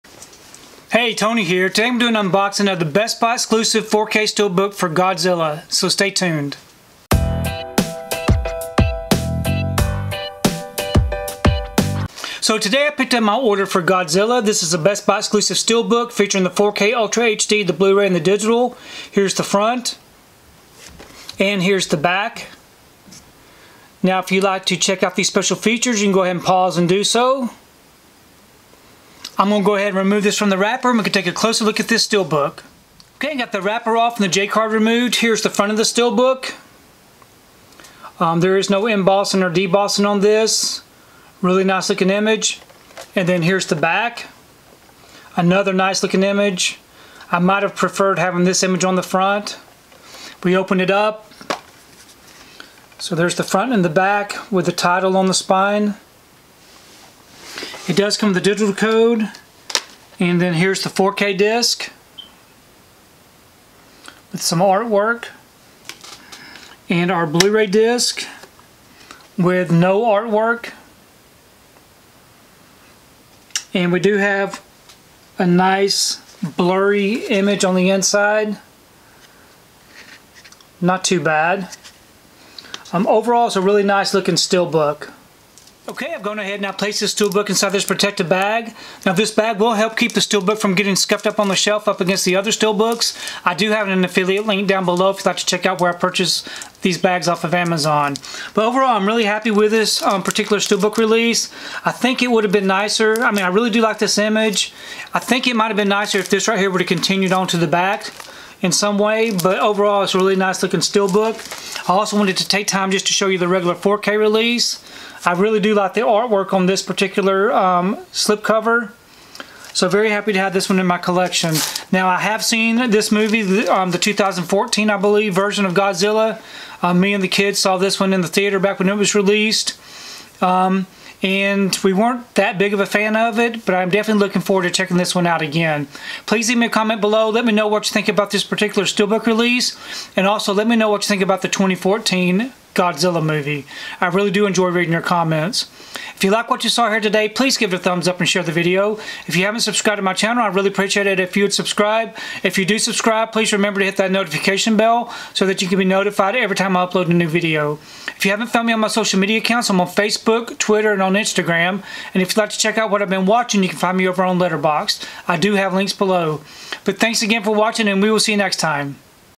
Godzilla (2014) Best Buy Exclusive 4K Ultra HD Blu-ray Steelbook Unboxing
Check out my unboxing of the Best Buy Collectible Steelbook for Godzilla (2014) 4K Ultra HD Blu-ray!

Purchase the 4K Ultra HD Blu-ray

These are the bags I use to protect my Steelbooks and Slipcovers: